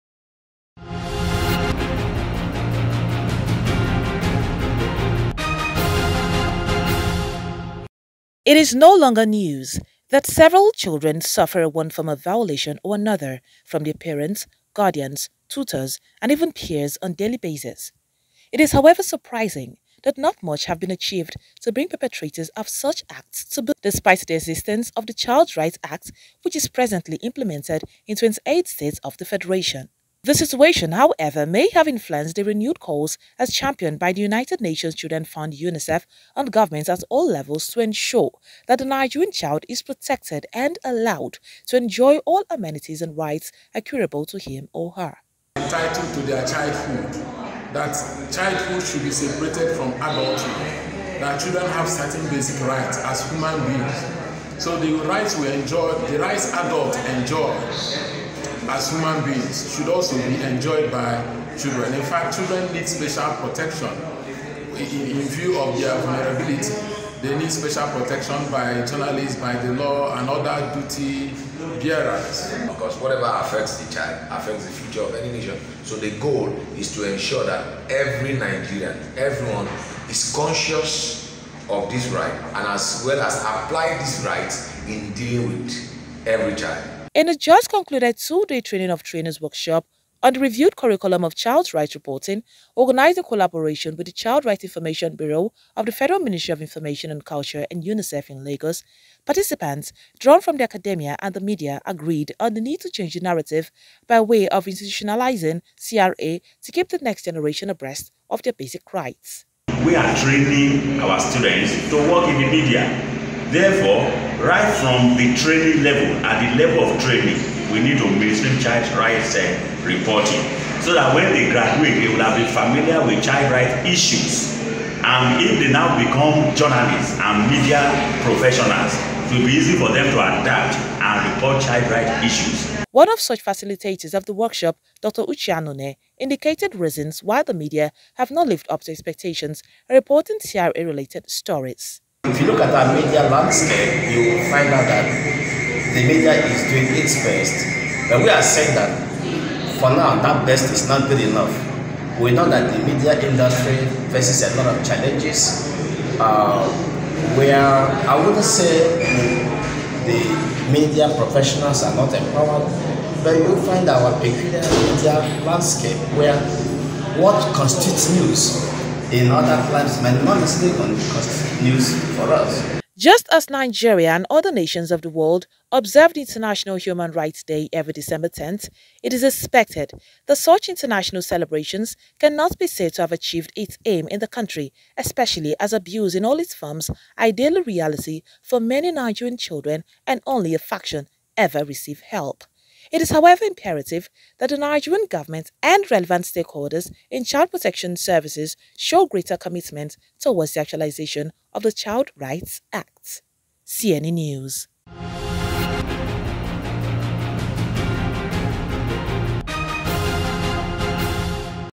Gains Of Mainstreaming Child Rights Reporting Into Curriculum.
It is nolonger news that several children suffer one form of violation or another from their parents, guardians, tutors and even peers on daily basis.

It is however suprising that not much have been achieved to bring perpetrators of such acts to book despite the existence of the child's rights act which is presently implemented in 28 states of the federation.

The situation however may have influenced the renewed call as championed by the United Nations Children Fund UNICEF on government at all levels to ensure that the Nigerian Child is protected and allowed to enjoy all amenities and rights accruable to him or her.

In a just concluded  2-day Training of Trainers Workshop on the Reviewed Curriculum of Child Rights Reporting” organised in collaboration with the Child Right Information Bureau of the Federal Ministry of Information and Culture and  UNICEF in Lagos, participants  drawn from the academia and the media agreed on the need to change the narrative by way of  institutionalizing  CRA to keep the next generation abreast of their basic rights.

However, one of the facilitators of the workshop Dr Uche Anune indicated reasons why the media have nor live up to expectation in reporting CRA related stories.

Just as Nigeria and other nations of the world observe the international Human rights day every December 10, it is expected that  such international celebrations cannot be said to have achieved it’s aim in the country especially as abuse in all it’s forms are a daily reality for many Nigerian children and only a faction ever receive help.

It is however imperative that the Nigerian government and relevant stakeholders in child protection services show greater commitment towards the actualisation of the Child Rights Act.